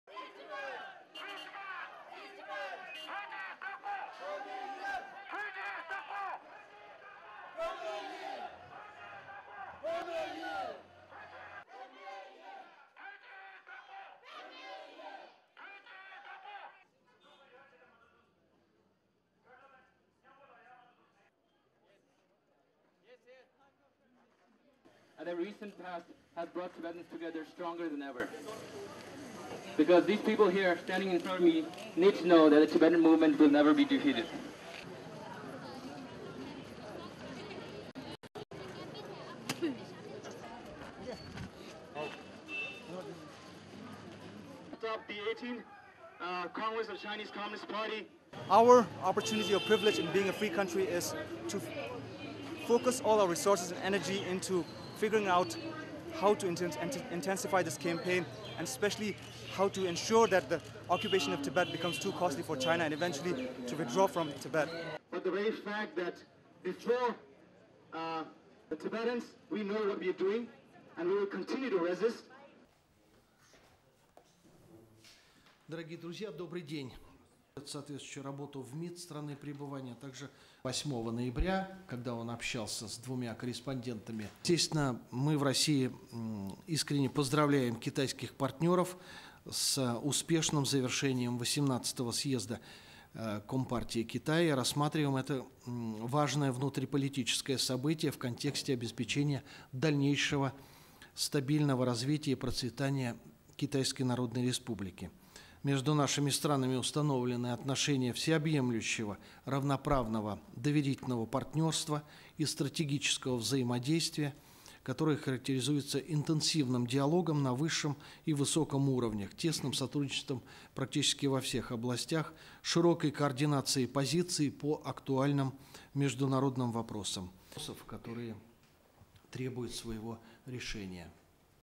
Tibetans and Russians react to new Chinese leadership
Dharmsala, India
1. Wide of Tibetans carrying Tibetan flags and shouting slogans during protest rally
2. Tibetans carrying Tibetan flags and a portrait of the Dalai Lama during the protest rally
3. Mid of Tibetans at rally
4. Close of an elderly Tibetan woman carrying a Tibetan flag
5. Tibetans wearing masks depicting the new politburo members of the Chinese Communist Party
6. SOUNDBITE: (English) Ugyen Badheytsang, Students for a Free Tibet (SFT), Canada
"The recent past has brought Tibetans together stronger than ever. Because these people standing in front of me need to know that Tibetan movement will never be defeated."
7. Mid of a Tibetan wearing mask depicting Xi Jinping, who leads the Politburo of the Chinese Communist Party
8. Various of Tibetans wearing masks depicting the new politburo members of the Chinese Communist Party
9. SOUNDBITE: (English) Tenzin Jigdal, Program Director of the Students for a Free Tibet (SFT), India
"Our opportunity of privilege of being in a free country is to focus all our energy into figuring out how to intensify this campaign, and especially how to ensure that the occupation of Tibet becomes too costly for China and eventually to withdraw from Tibet."
10. Various of Tibetans wearing masks depicting the new politburo members of the Chinese Communist Party
Moscow, Russia
11. Alexander Lukashevich, spokesman of the Russian Foreign Ministry, entering press briefing
12. Cutaway of journalists
13. Wide of briefing
14. SOUNDBITE: (Russian) Alexander Lukashevich, spokesman of the Russian Foreign Ministry:
"Naturally, we in Russia sincerely congratulate our Chinese partners on the successful completion of the XXVIII Congress of the Chinese Communist Party and view this important political event in the life of their country in the context of further consolidation of the stable development and prosperity of the Chinese People's Republic. Between our countries we have established comprehensive relations of strategic partnership based on trust and equality which are characterised by an intensive dialogue on the highest and high levels and close cooperation in practically all spheres and wide coordination on international issues."
15. Wide of briefing
STORYLINE:
Tibetan exiles in the Indian hill town of Dharmsala took to the streets on Thursday to demand that the new leadership of the Chinese Communist Party acknowledge the failure of its policy in Tibet.
Tibet support groups overseas say the increase in protests in recent days is meant to highlight Tibetan unhappiness with Chinese rule as the country's leaders handed over power to younger successors at a party congress in Beijing.
On Thursday, dozens of protesters, including members of student groups, marched in Dharmsala carrying Tibetan flags and shouting slogans such as "What do we want?  We want freedom', and "Tibet is for Tibetans".
Afterwards they assembled at the town square alongside protesters wearing masks depicting Xi Jinping, who was elected general secretary of the ruling Communist Party of China on Thursday, and other politburo members.
Addressing the crowd through a loudhailer, Ugyen Badheytsang of the Students for a Free Tibet (SFT) group from Canada said: "The recent past has brought Tibetans together stronger than ever. Because these people here standing in front of me need to know that the Tibetan movement will never be defeated."
Xi Jinping, who leads the Politburo of the Chinese Communist Party, is set to take over from Hu Jintao under whose leadership Tibetan discontent with the Chinese rule became more visible.